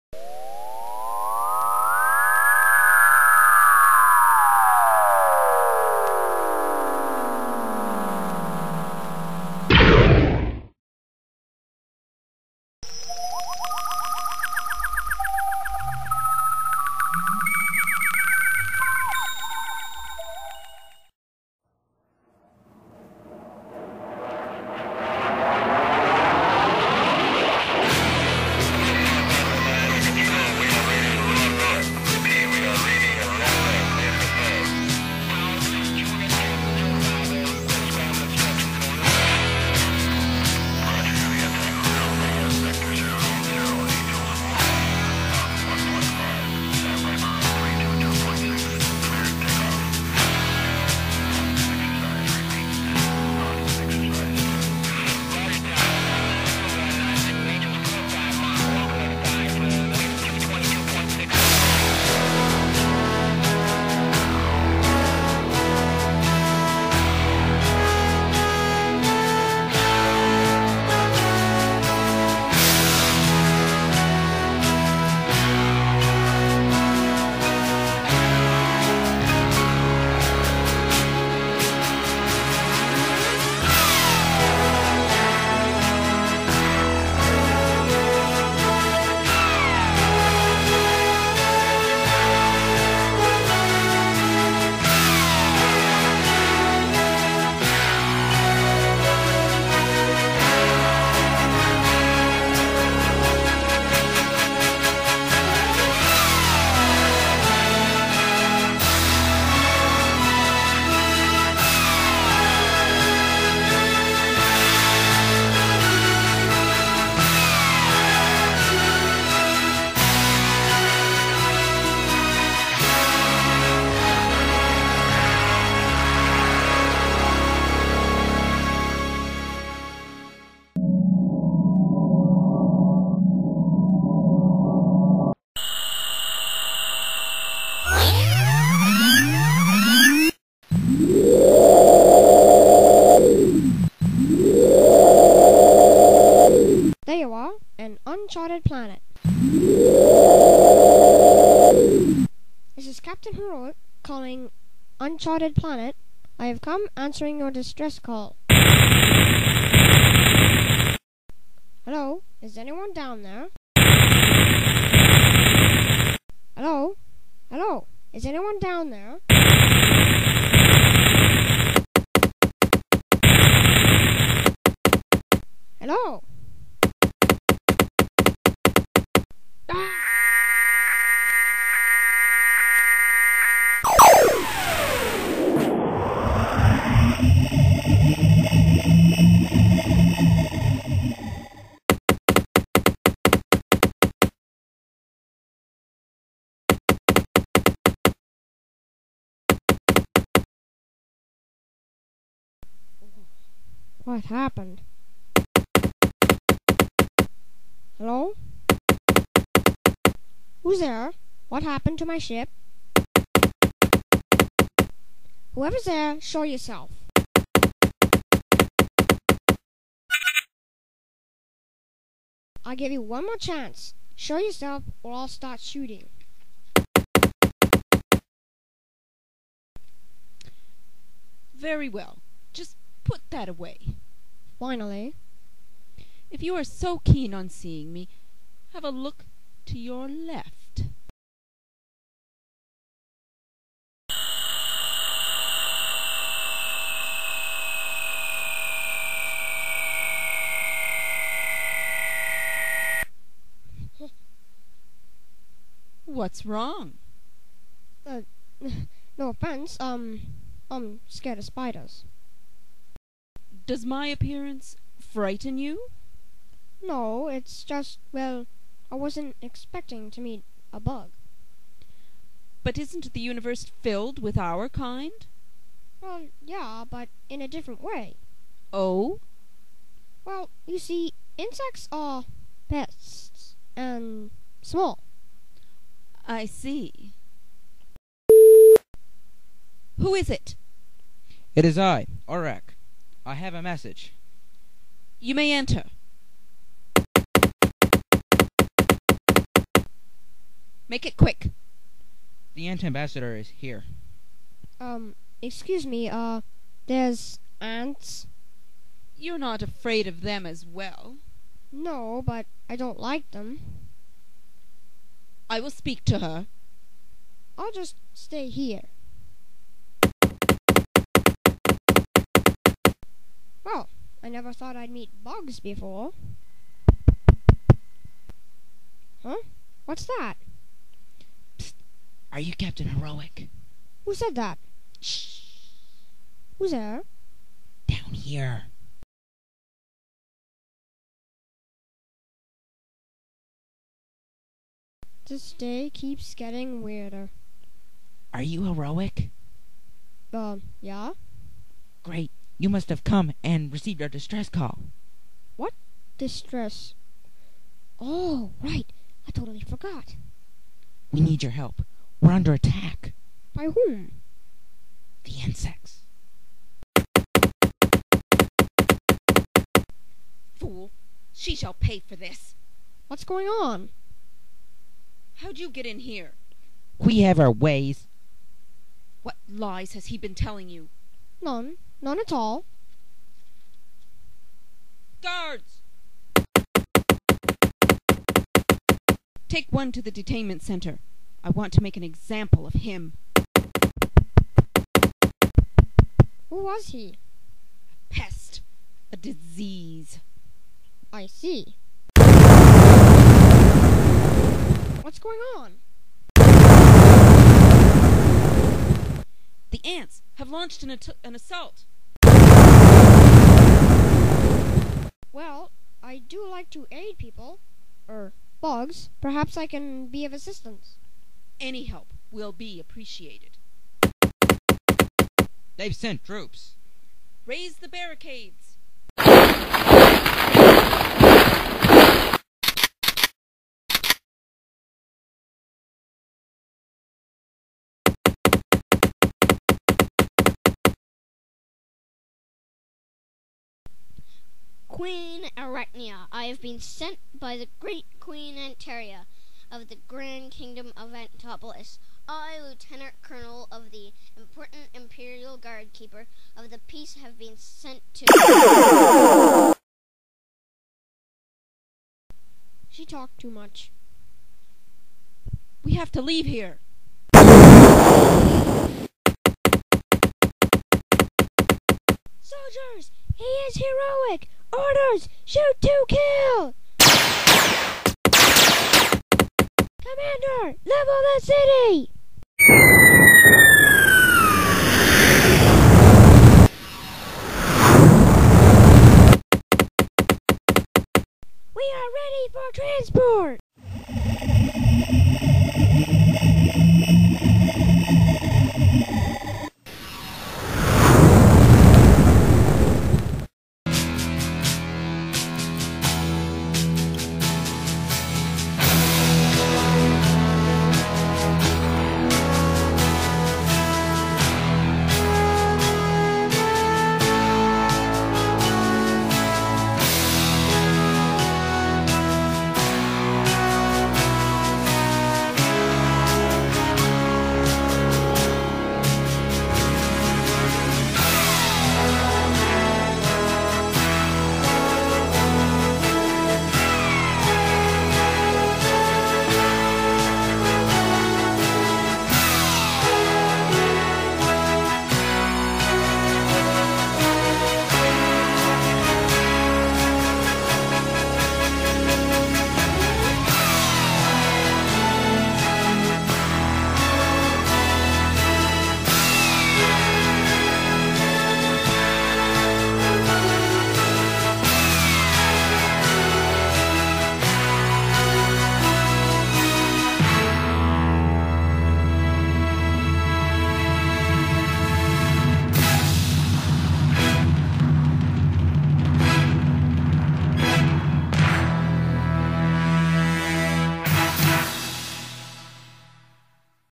Captain Heroic: Episode 2 Planet of Insects
Answering a distress call from an uncharted planet, Captain Heroic is taken captive by a group of giant insects!

Made in 2005/2006 with Anim8or